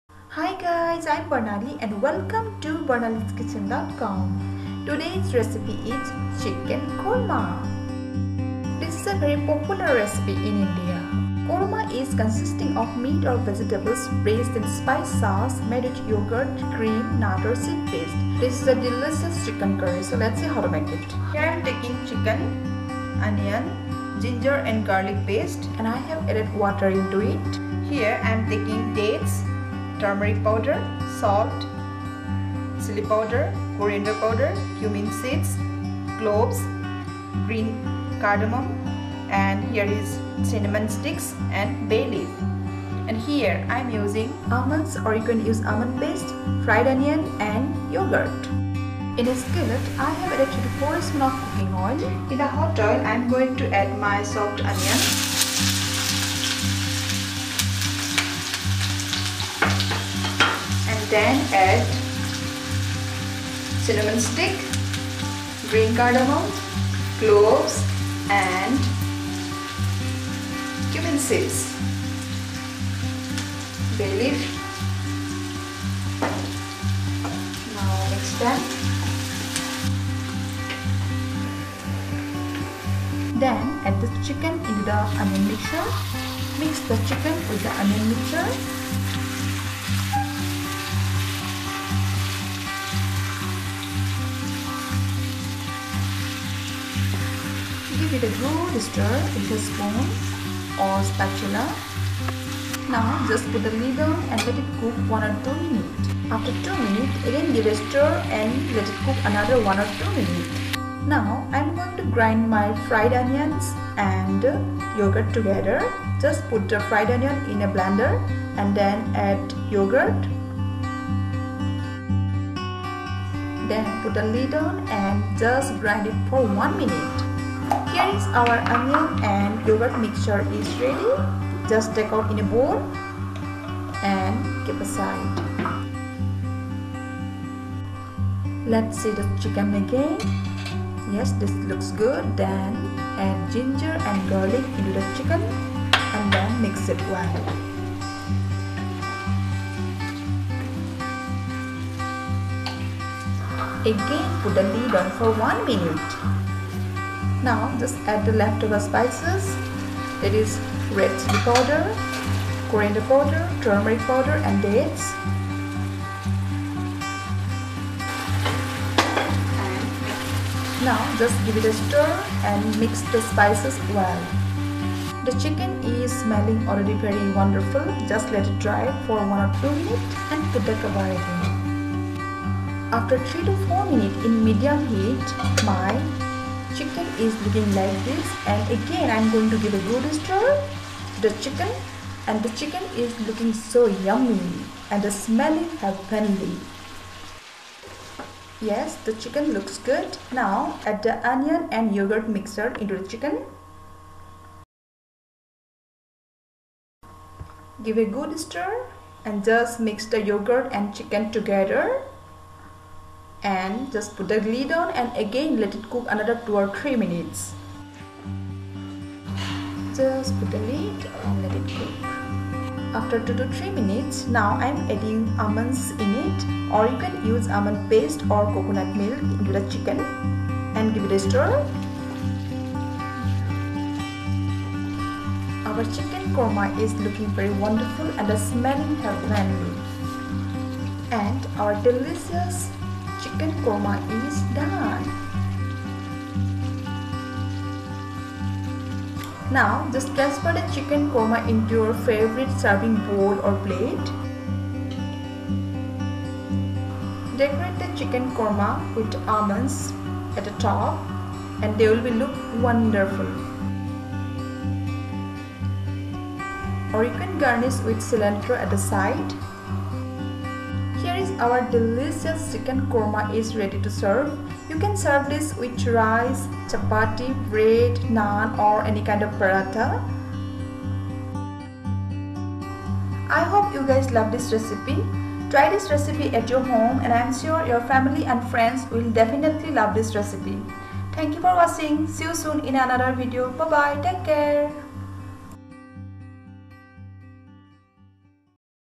Chicken Korma Recipe - Chicken Korma| Delhi Style Chicken Korma Recipe | Restaurant Style
Korma  spelled kormaa, qorma, khorma, or kurma, is a dish originating in Central Asia consisting of meat or vegetables braised in a spiced sauce made with yogurt, cream, nut or seed paste.Chicken korma is simmered in a creamy fragrant sauce with onion, garlic, ginger and almonds. Serve over basmati or sticky rice or naan, paratha.

Ingredients:

Ginger garlic paste                            1 spoon (I have added water )
Almond or poppy seed paste
Clove                                                     5-6
Bay leaf                                                 2
Cardamom                                           4-5
Cinnamon sticks                                 2
Dates                                                    1

Method:
In a pan add 4-5 spoon of cooking oil and let it hot.once the oil is hot then add chopped onion, cardamom,cinnamon sticks ,cloves, bay leaf and cumin seeds.Fry it for few second or until the onion looks translucent. Then add the chicken into the onion mixture and give it good mix.Then put the lid on and let it cook for few minutes.
Keep stirring  between.Now grind the fried onion and yogurt together.In a blender add the fried onion and yogurt together and grind it for few second.Lets check the chicken again.Add the ginger garlic paste into the chicken and let it cook.After few minutes add the left over dry spices in the chicken and give it good stir.After 4-3 minutes in medium  flame  now add the onion and the yogurt mixture into the chicken.Mix them well and again let it cook another 2-3 minutes.After few minutes add the whole almonds or poppy seeds paste , or you can add coconut milk into the chicken.Our delicious chicken korma is ready.Serve this yummy chicken korma with rice, chapati, roti, bread , naan or paratha.Try this wonderful chicken korma at your home and i am sure your family and friends will definitely love it.Enjoy...

(use key word barnali's Kitchen